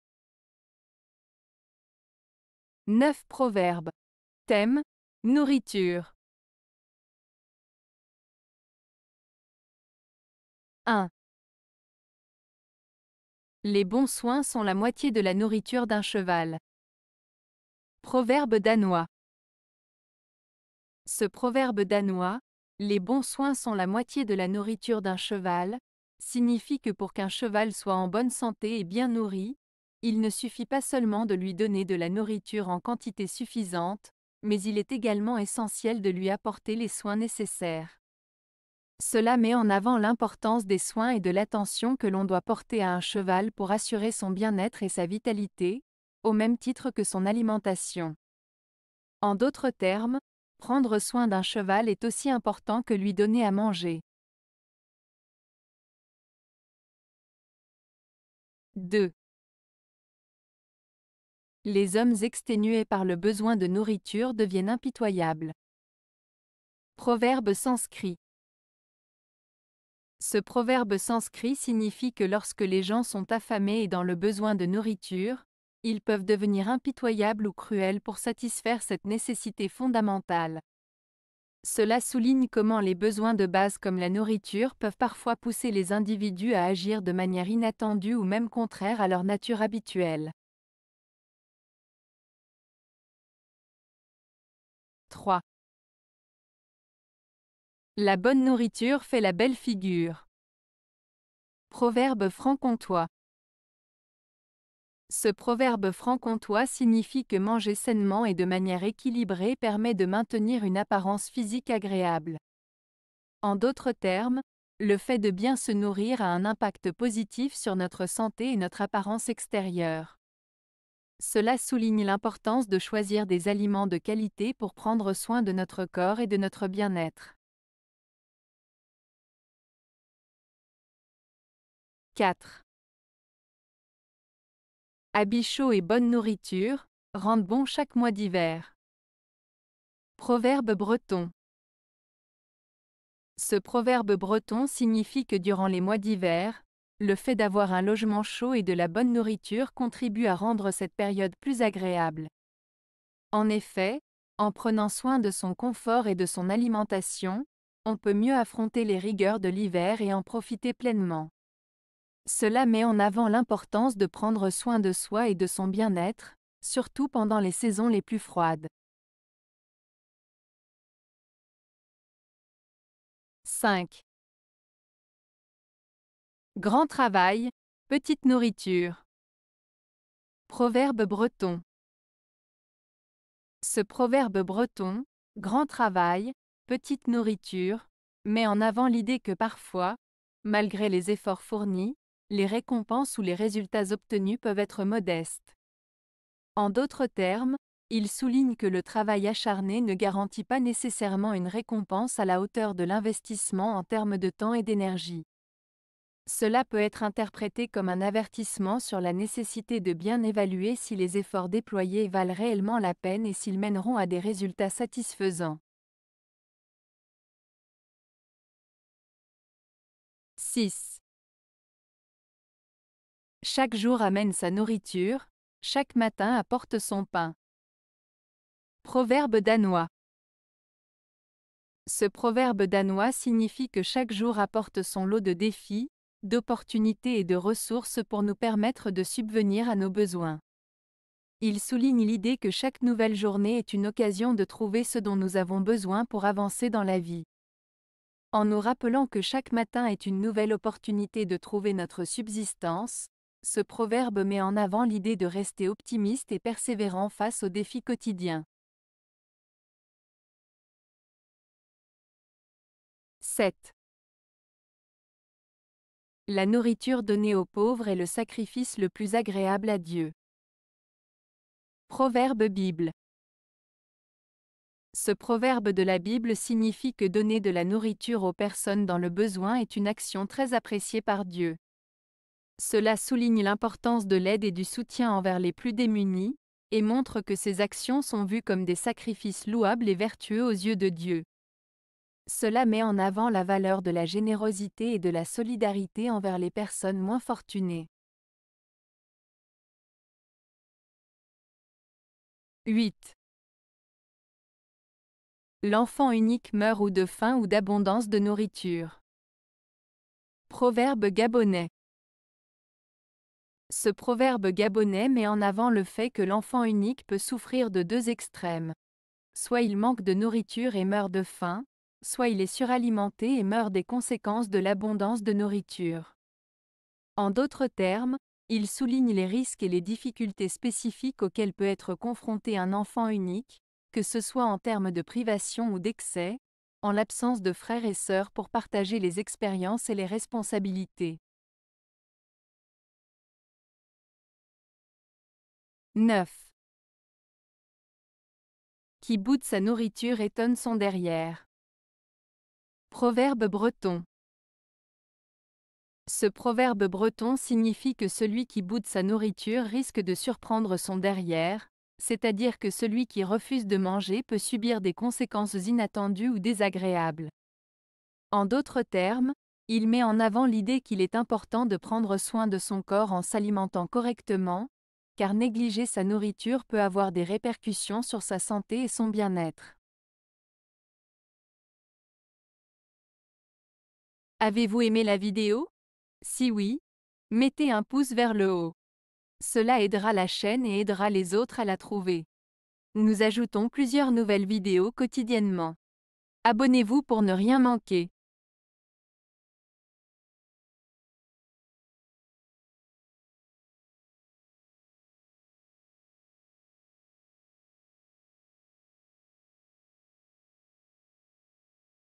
Proverbes - Nourriture (1 de 3)
00:00 Introduction
00:08 Proverbe Danois - Les bons soins sont la moitié de la nourriture d'un cheval.
00:56 Proverbe Sanskrit - Les hommes exténués par le besoin de nourriture deviennent impitoyables.
01:32 Proverbe Franc-comtois - La bonne nourriture fait la belle figure.
02:09 Proverbe Breton - Habit chaud et bonne nourriture, rendent bon chaque mois d'hiver.
02:50 Proverbe Breton - Grand travail, petite nourriture.
03:37 Proverbe Danois - Chaque jour amène sa nourriture, chaque matin apporte son pain.
04:20 Proverbe Bible - La nourriture donnée aux pauvres est le sacrifice le plus agréable à Dieu.
05:00 Proverbe Gabonais - L'enfant unique meurt ou de faim ou d'abondance de nourriture.
05:43 Proverbe Breton - Qui boude sa nourriture étonne son derrière.